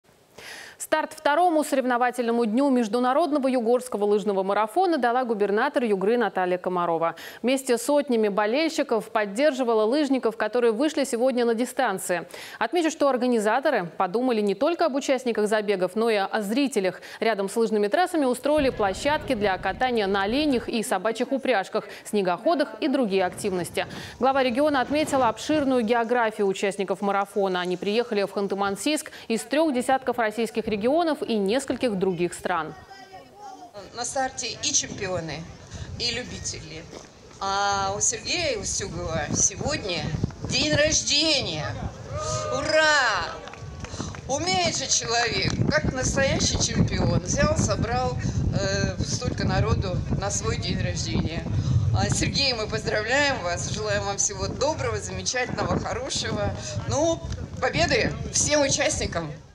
«Собрал столько народа в день рождения», - Наталья Комарова поздравила Устюгова перед забегом
Старт второму соревновательному дню международного Югорского лыжного марафона дала губернатор Югры Наталья Комарова.